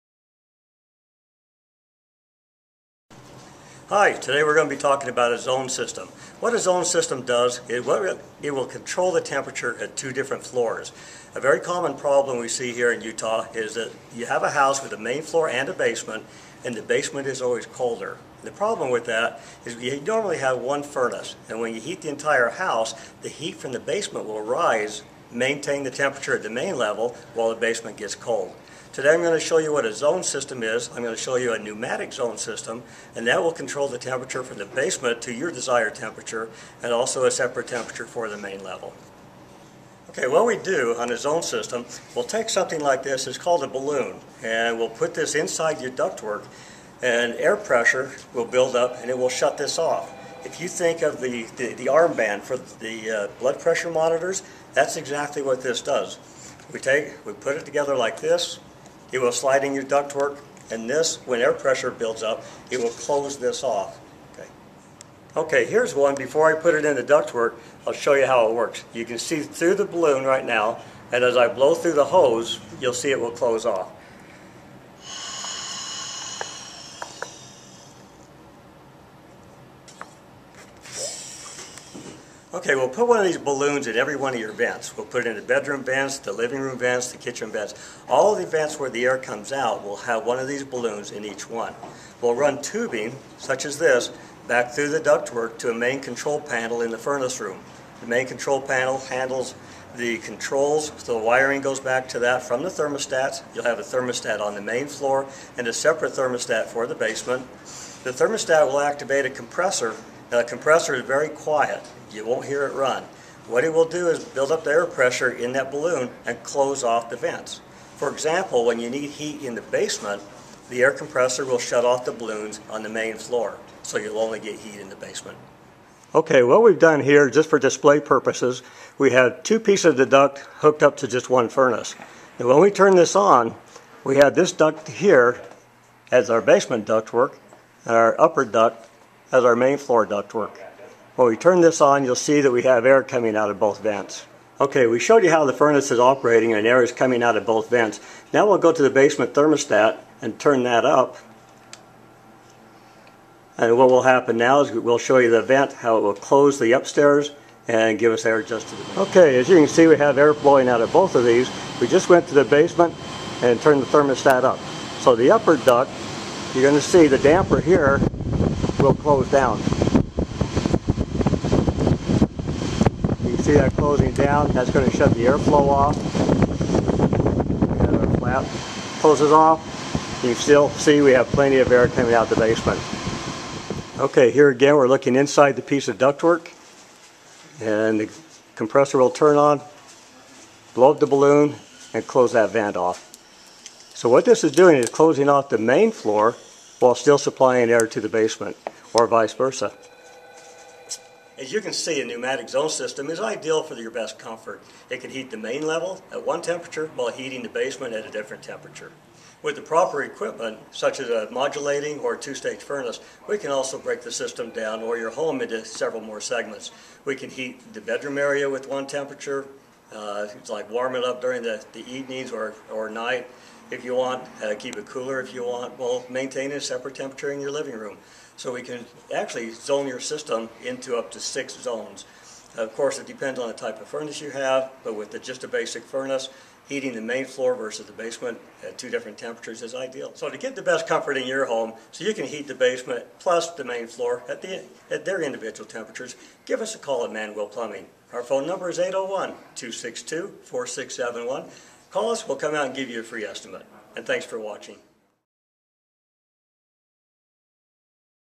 How to Control Temperatures on Different Floors with one System | Manwill
Excellent zoning system for a retrofit application. The ideal way to control temperatures on different floors to your desired setting and do so with only one furnace or air conditioner. So, if you only have one furnace, your basement is cooler than the main floor, and you only need to add heat to the basement, this is the system for you.  Or, if you need to cool an upper level without sending any cool air to the main floor, it works for this application, too.